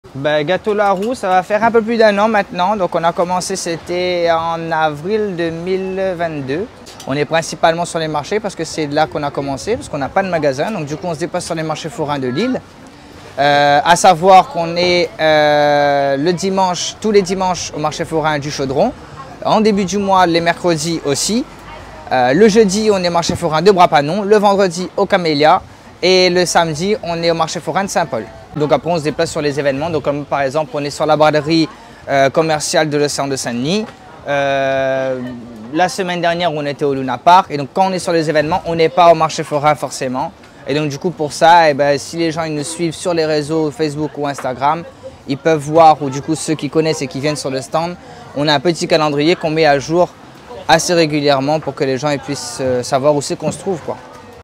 Pause gourmande : quand le « gato la rou » taïwanais rencontre les saveurs de La Réunion
Oú déguster les gato la rou